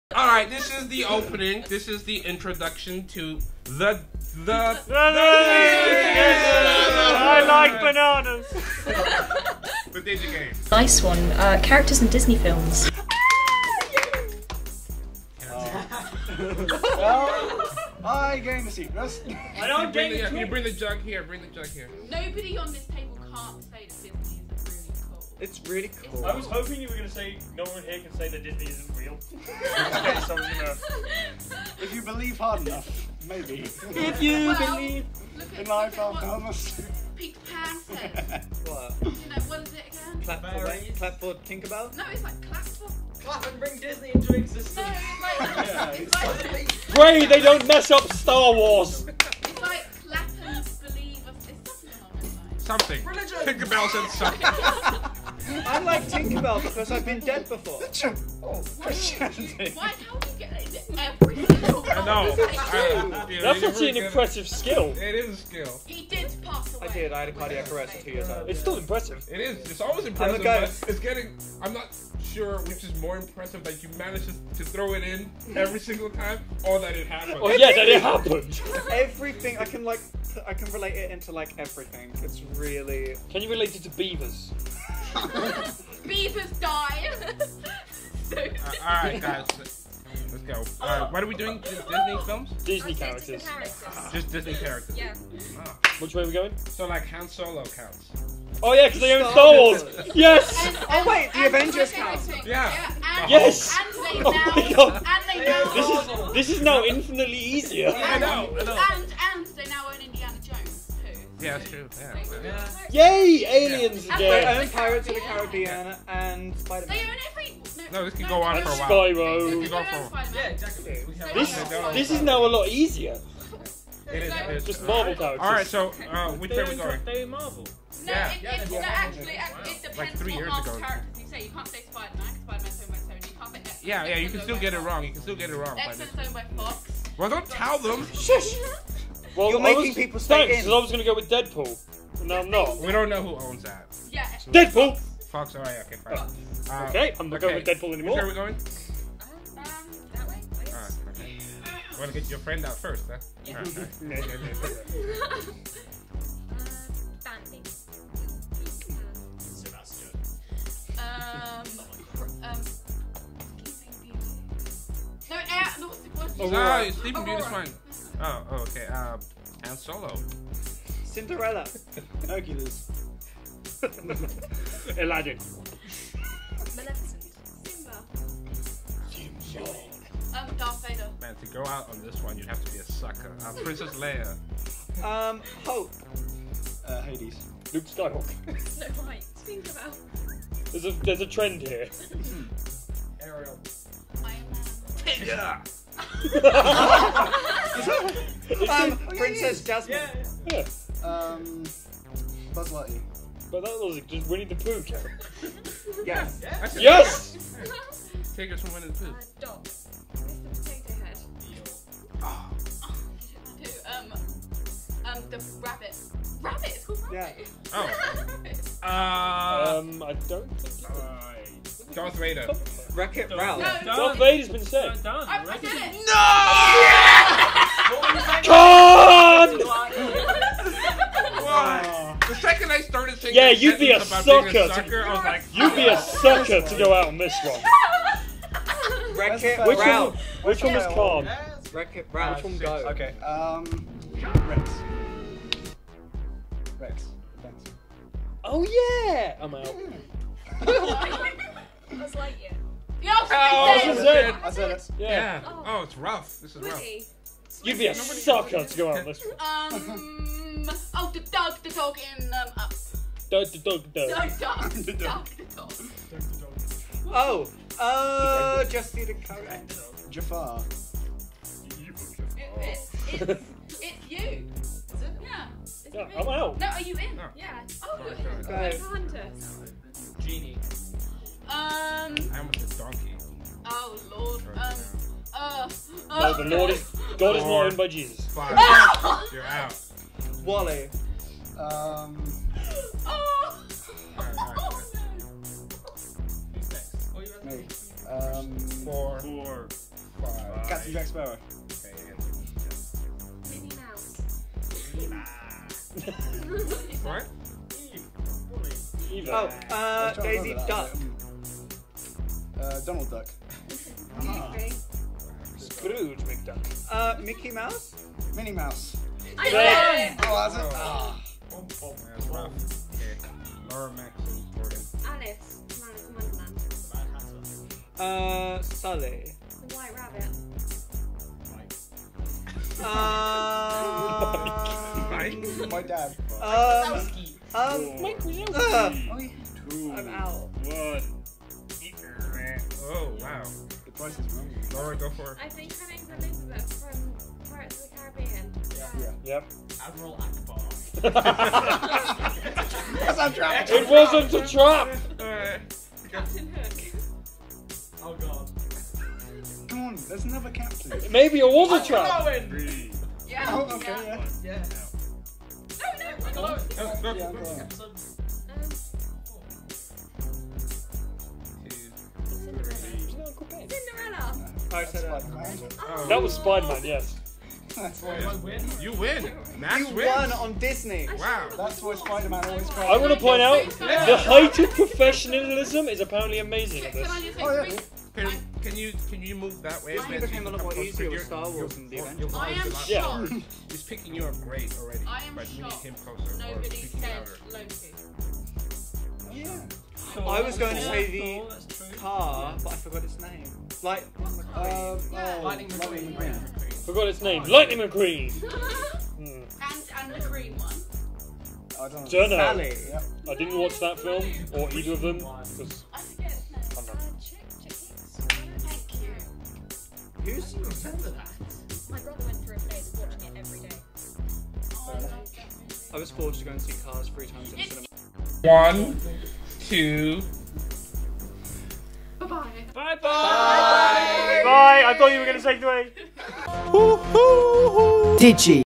The Digi Game Show - Disney Characters
Contestants use their knowledge in battle. Feel creative, be inspired. Friday nights at Norden Farm.